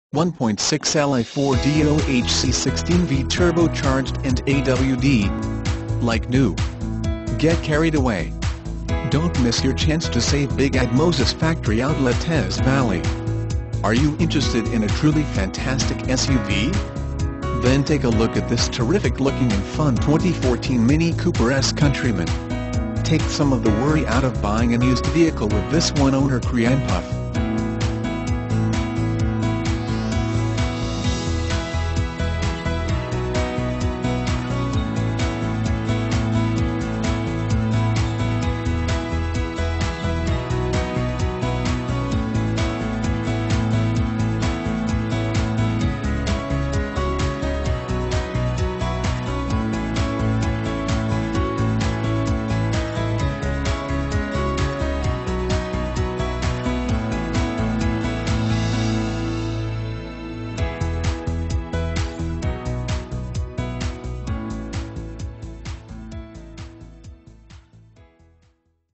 2014 MINI COOPER COUNTRYMAN Hurricane, WV OW15011
2014 MINI COOPER COUNTRYMAN Hurricane, WV
Stock# OW15011

Moses Factory Outlet Teays Valley
700 Liberty Park Dr.

1.6L I4 DOHC 16V Turbocharged and AWD. Like new. Get carried away. Don't miss your chance to save big at Moses Factory Outlet Teays Valley. Are you interested in a truly fantastic SUV? Then take a look at this terrific-looking and fun 2014 Mini Cooper S Countryman. Take some of the worry out of buying an used vehicle with this one-owner creampuff.